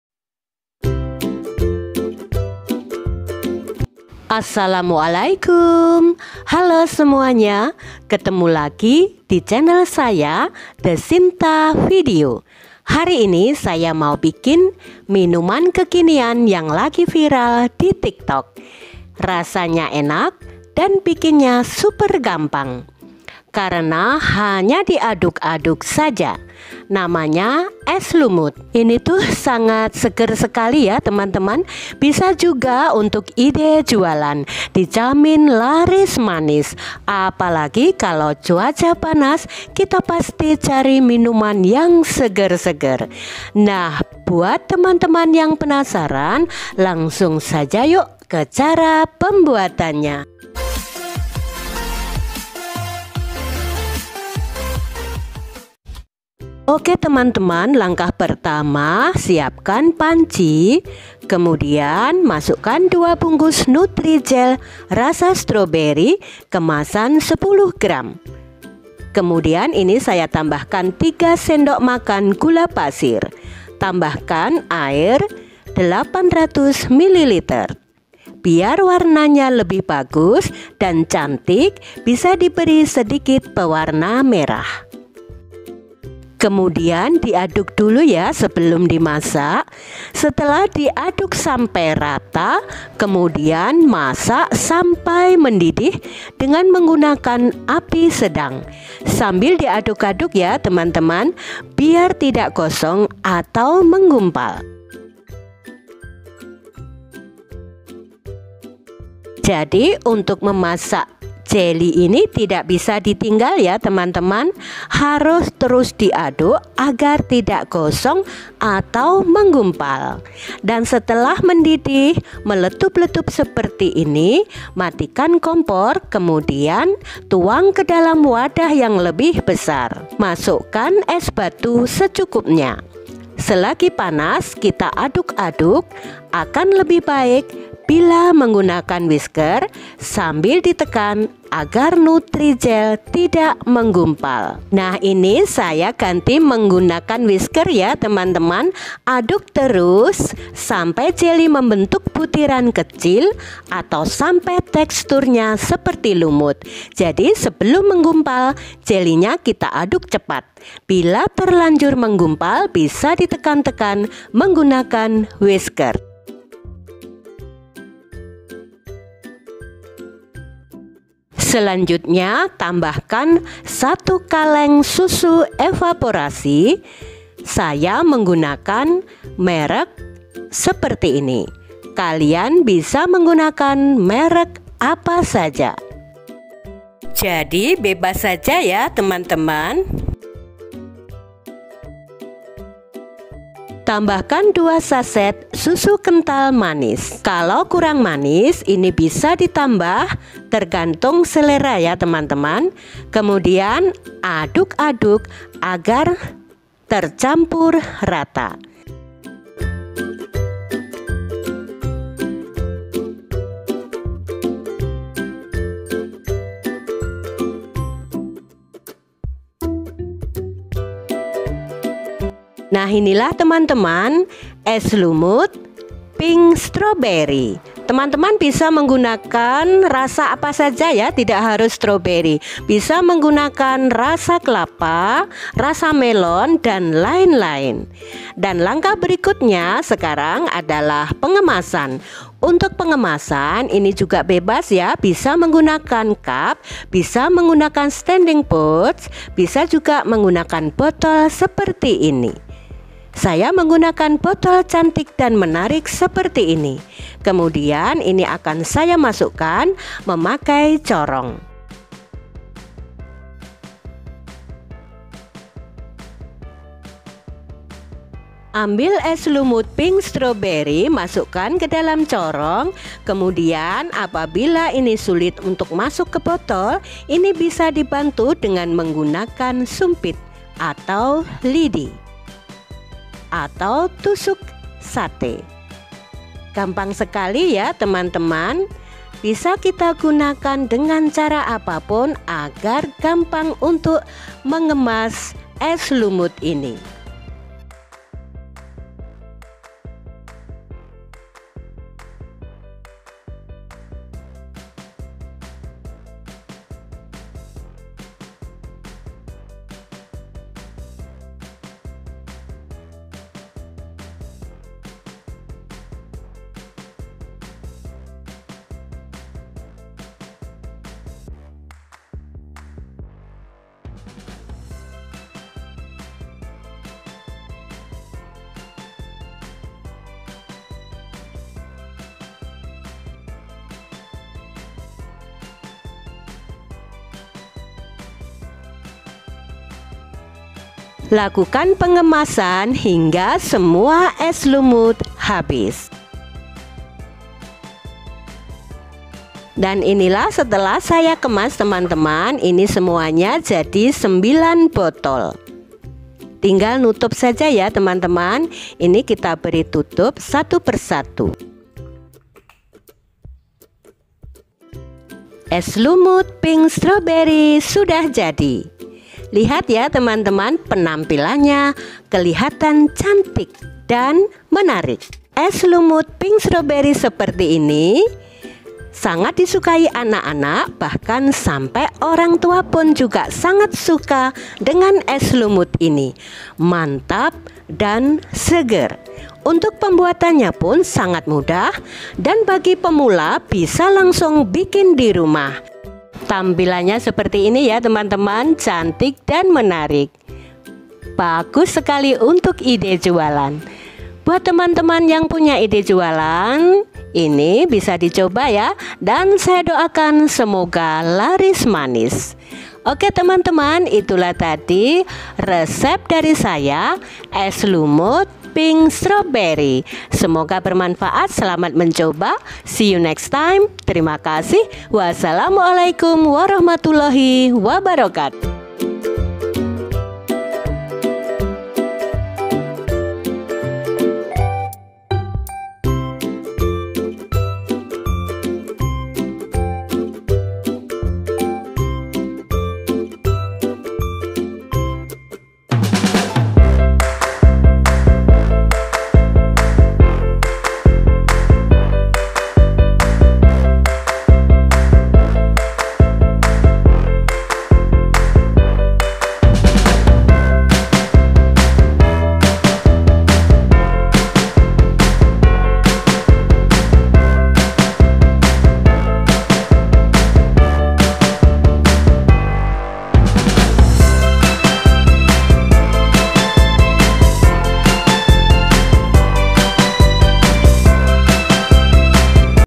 Hanya Diaduk Bisa Untung Besar‼️Es Lumut Viral Tik Tok || Ide Jualan
Hanya Diaduk Bisa Untung Besar‼️Es Lumut Viral Di Tik Tok || Ide Jualan

Bahan
2 sachet nutrijel strawberry
3 sdm gula pasir
1 kaleng susu evaporasi
2 sachet SKM
800 ml air
pewarna makanan merah
es batu

es, lumut, es lumut, minuman es lumut, minuman viral, minuman viral di tiktok, es lumut viral, minuman kekinian